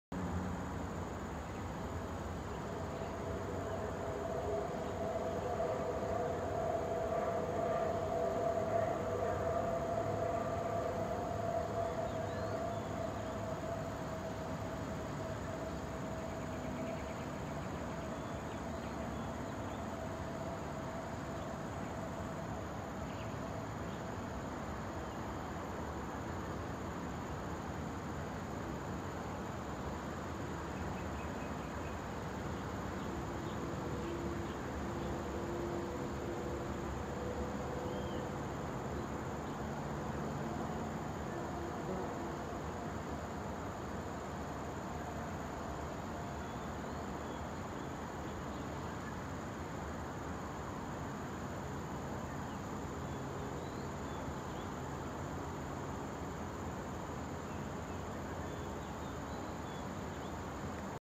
2018-11-20: Clear but cloudy Morning skyline of Kuala Lumpur
Mid morning 10am clear skyline of Kuala Lumpur. MRT trains seen passing by the rain forest and concrete jungle of Kuala Lumpur. Birds chirping in the nature.